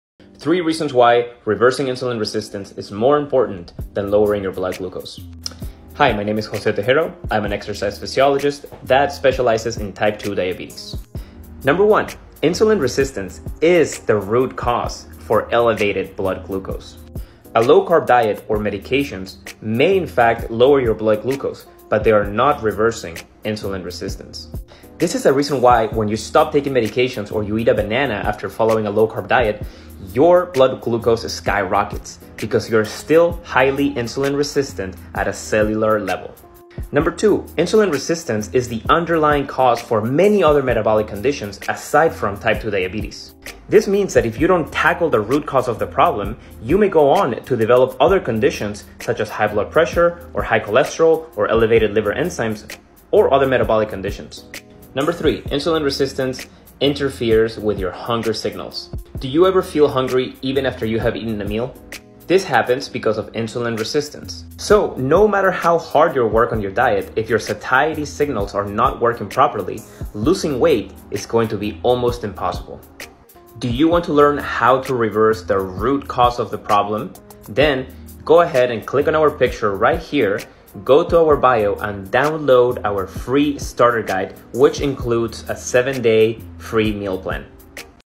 3 Reasons Why Reversing Insulin Resistance is More Important Than Lowering Your Blood Glucose
Reversing insulin resistance is more important than lowering your blood glucose